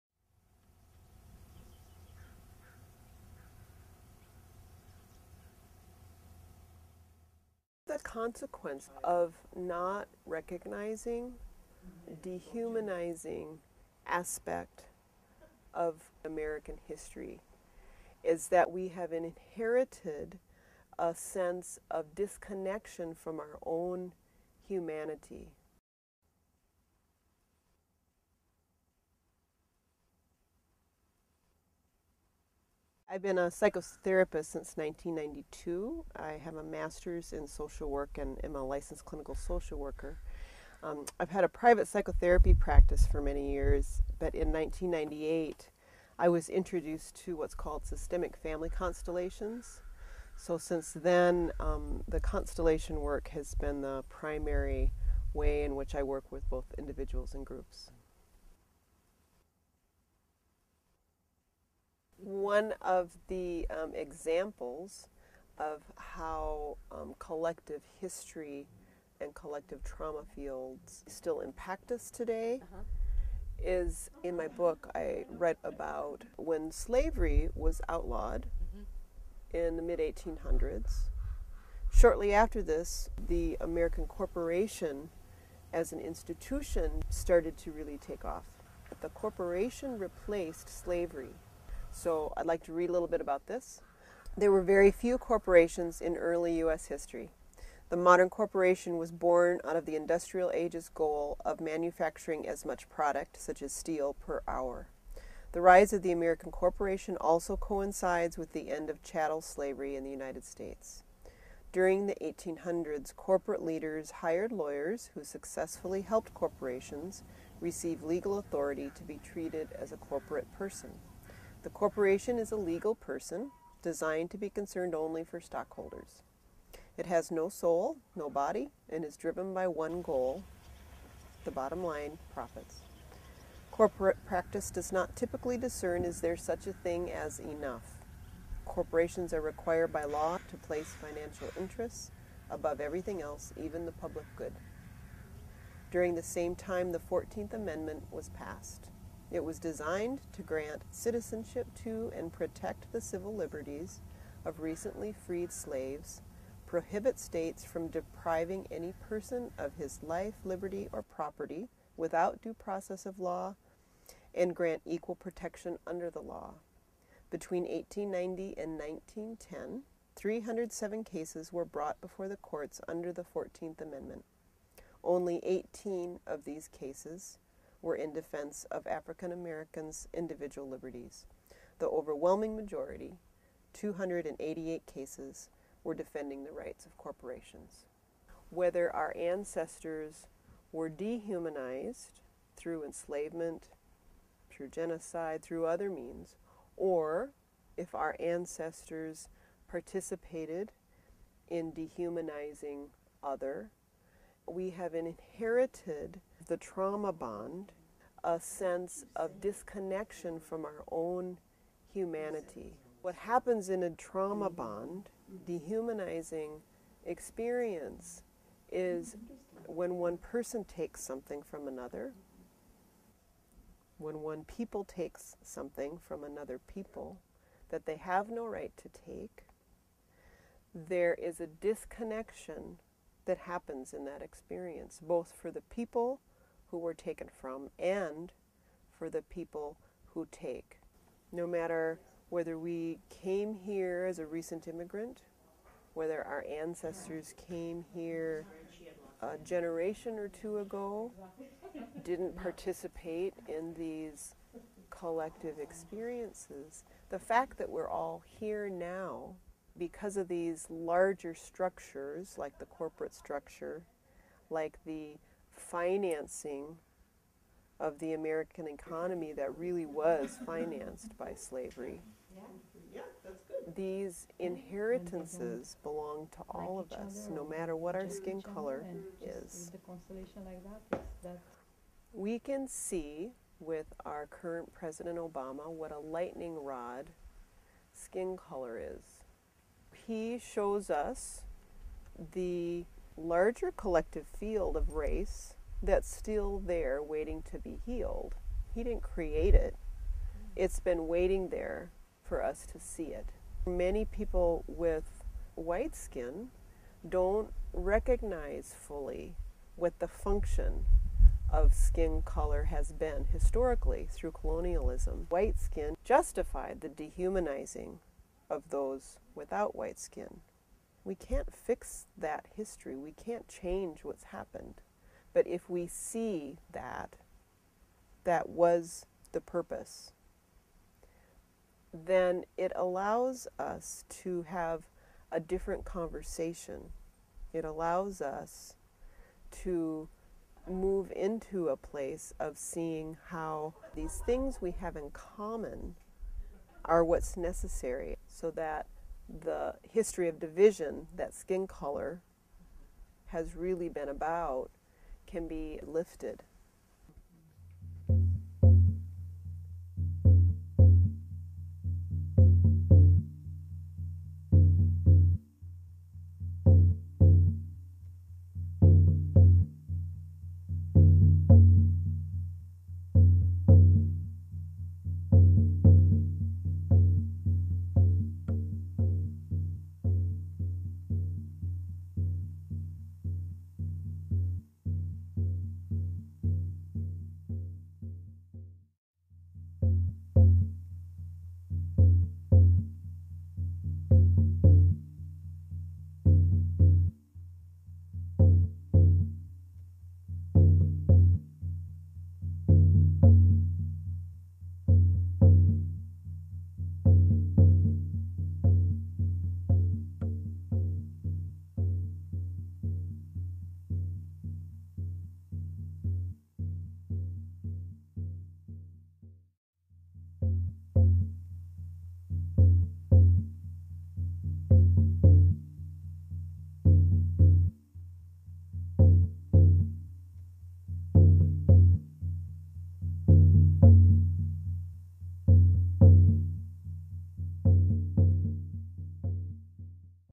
Lisa Iversen on Current Impacts of America's Colonial Past
Filmmaker Regan Johnson spent a day in August 2012 taping a Systemic Constellation circle led by Lisa Iversen Regan produced four videos from that experience, shedding light on the connection between the Systemic Constellation method and healing the colonial past of the United States for all Americans today...and tomorrow.

Thank you to all those who participated in these videos, including Sabine and Dan Olsen, Kate Regan, Simona Draghici, and Richard Lemaire.